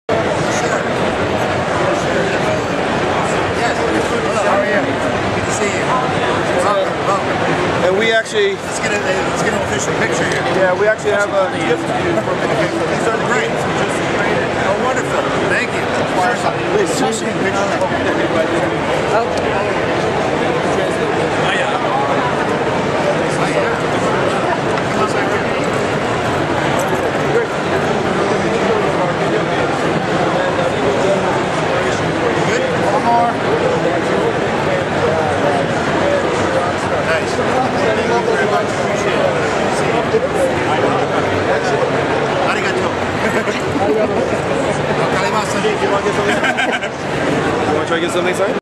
Keiji Inafune Meets Bruce Campbell
The masters of the zombie genre, Mr. Inafune and Evil Dead's Bruce Campbell, met up at New York Comic Con!